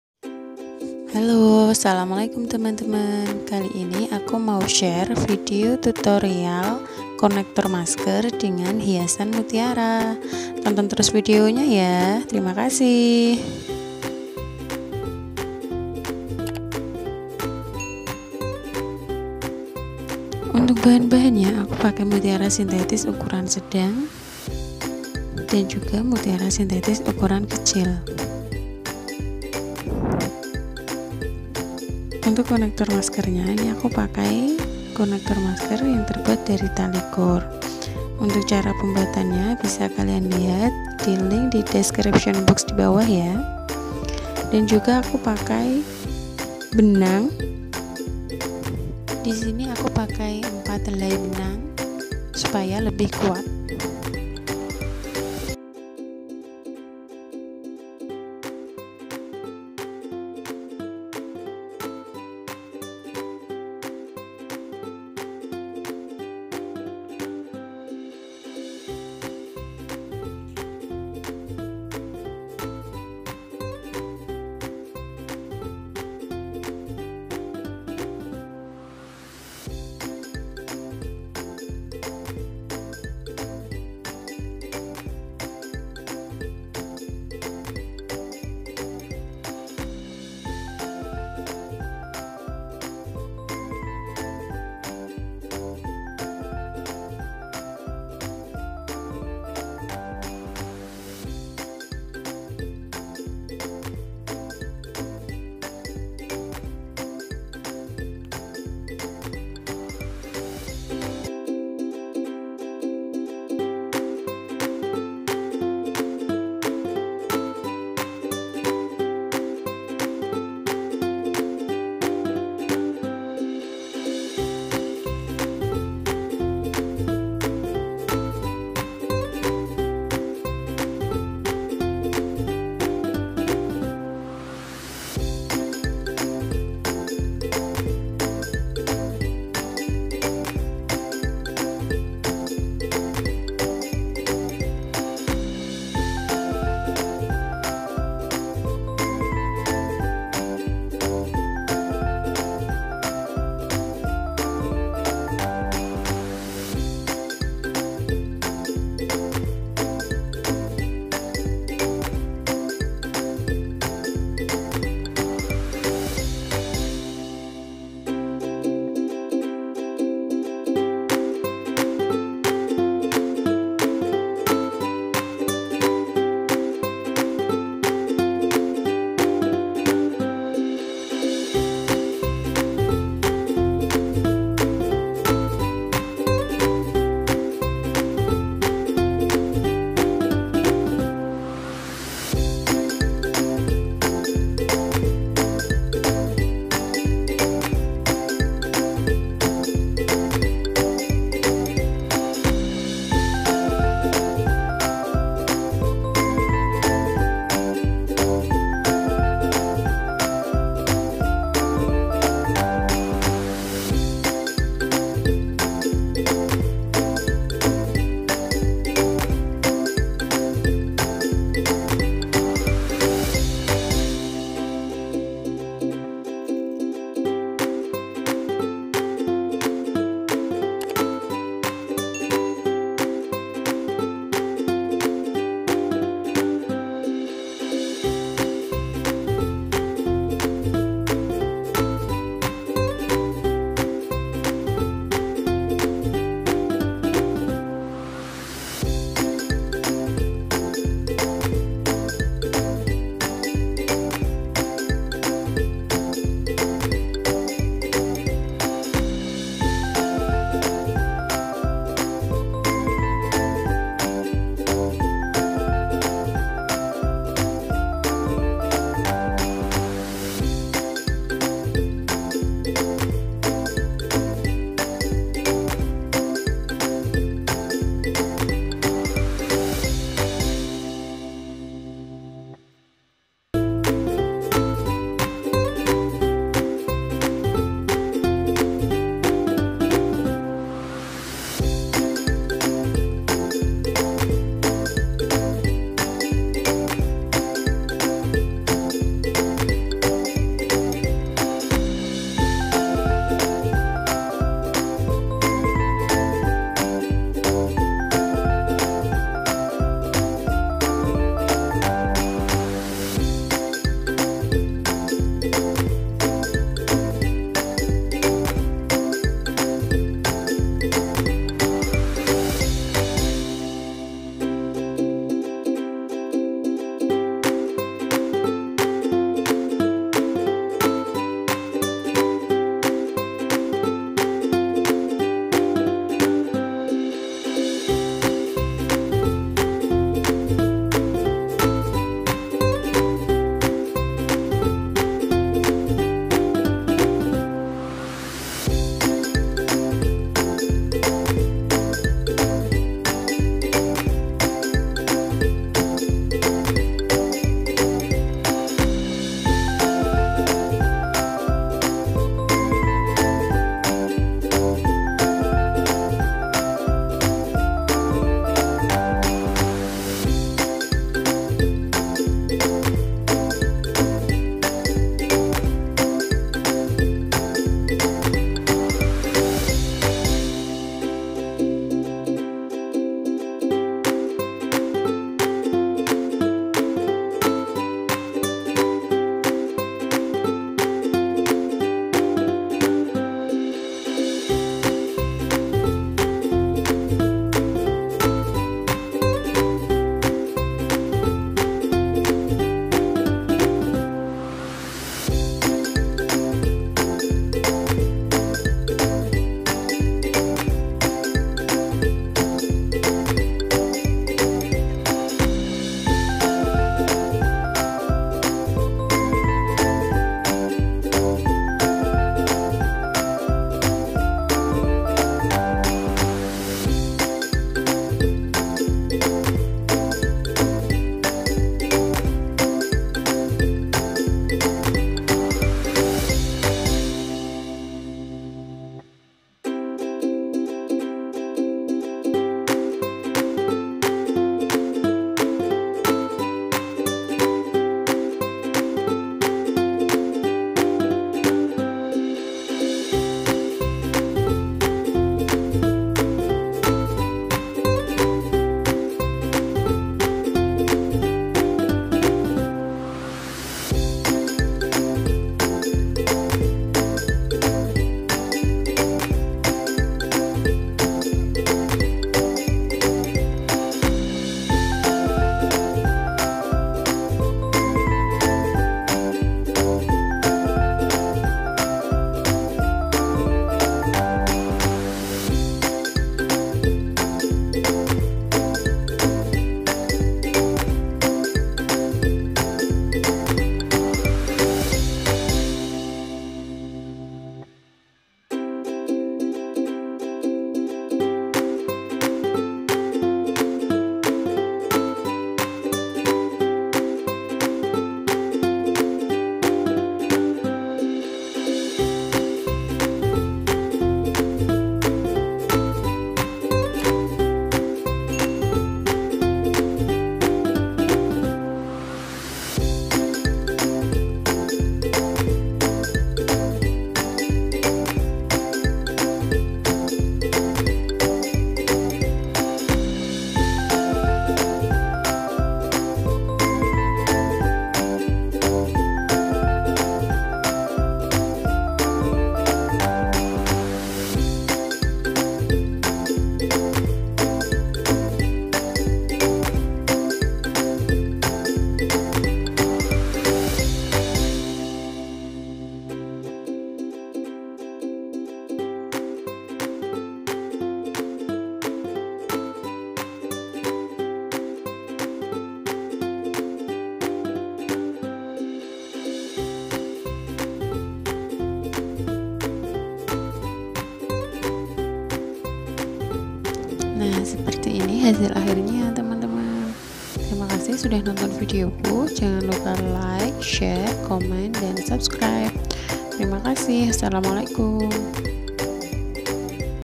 Mask connector tutorial with pearl decoration | Craft by Kholifa
Mask connector tutorial with pearl

Hello friends, this time I will share a mask connector tutorial with pearl decoration. Watch the video until it's finished. Follow the steps from the beginning